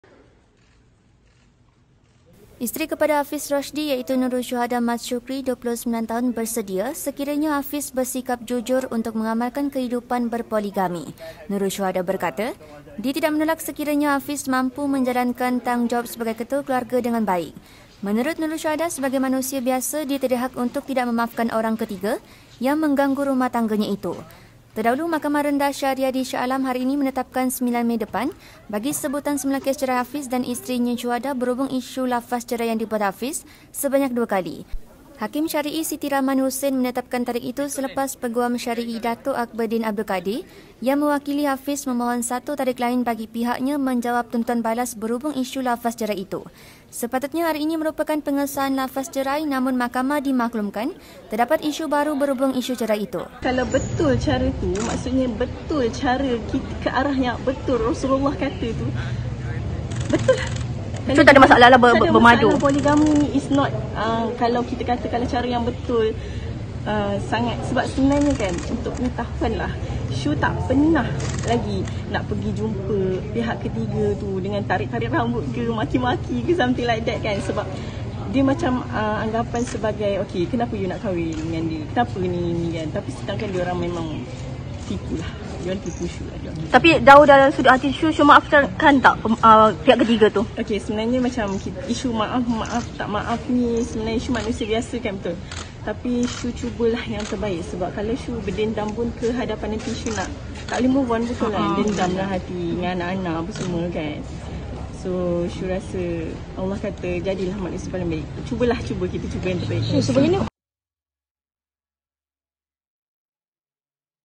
Tak kisah bermadu jika Hafidz jujur - Nurul Shuhada
SHAH ALAM: Isteri kepada Hafidz Roshdi, Nurul Shuhada Mat Shukri, 29, terbuka bermadu sekiranya suaminya bersikap jujur untuk berpoligami.